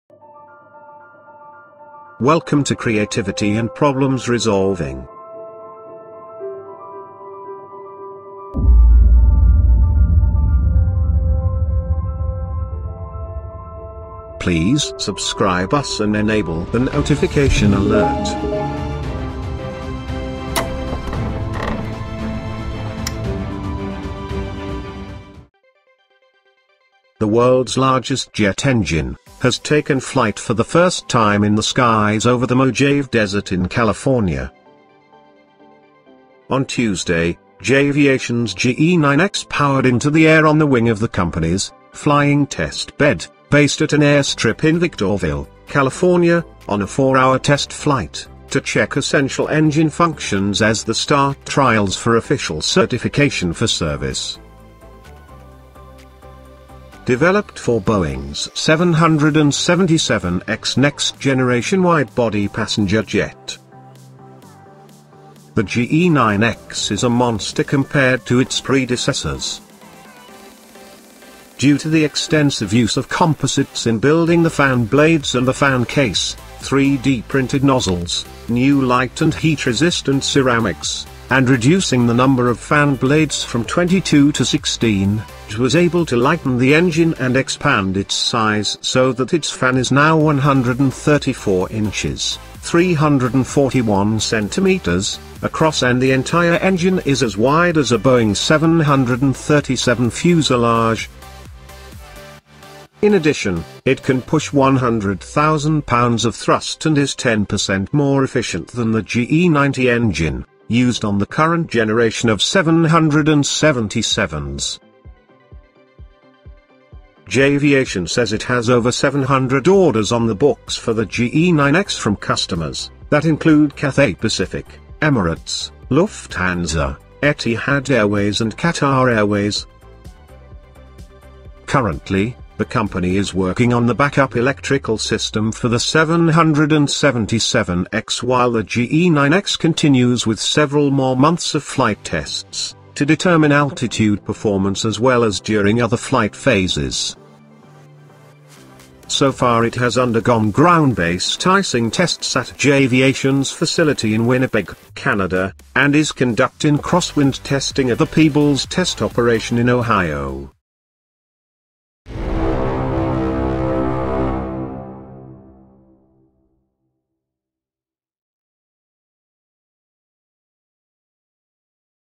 The World’s Largest Jet Engine GE9X First Flight 2018
The world's largest jet engine, has taken flight for the first time in the skies over the Mojave Desert in California. On Tuesday, GE Aviation's GE9X powered into the air on the wing of the company's, "Flying Test Bed", based at an airstrip in Victorville, California, on a four-hour test flight, to check essential engine functions as the start trials for official certification for service.

Developed for Boeing's 777X next-generation wide-body passenger jet. The GE9X is a monster compared to its predecessors. Due to the extensive use of composites in building the fan blades and the fan case, 3D-printed nozzles, new light- and heat-resistant ceramics, and reducing the number of fan blades from 22 to 16, GE was able to lighten the engine and expand its size so that its fan is now 134-inches (341 cm) across and the entire engine is as wide as a Boeing 737 fuselage. In addition, it can push 100,000 lb of thrust and is 10 percent more efficient than the GE90 engine, used on the current generation of 777s.

GE Aviation says it has over 700 orders on the books for the GE9X from customers, that include Cathay Pacific, Emirates, Lufthansa, Etihad Airways and Qatar Airways. Currently, the company is working on the backup electrical system for the 777X while the GE9X continues with several more months of flight tests, to determine altitude performance as well as during other flight phases. So far it has undergone ground-based icing tests at GE Aviation's facility in Winnipeg, Canada, and is conducting crosswind testing at the Peebles Test Operation in Ohio.